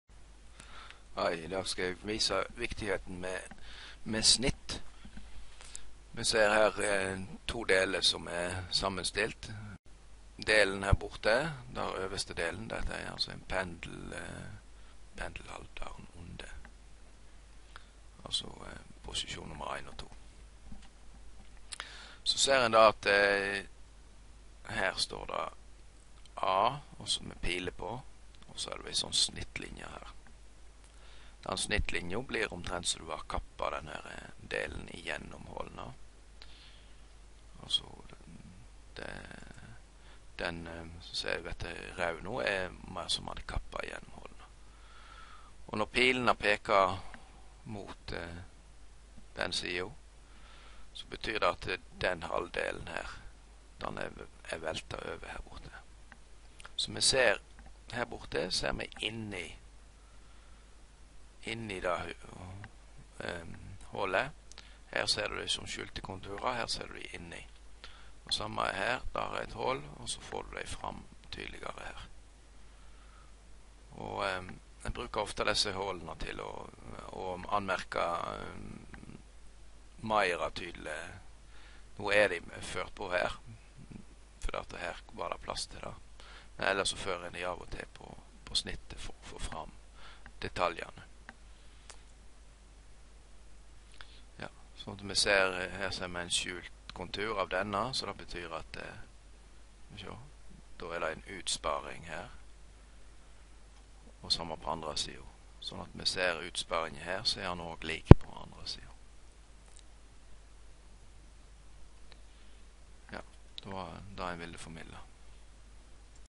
Bruk av Snitt på Maskinteikning
Når brukar ein snitt, kva er det og kva fordeler er det.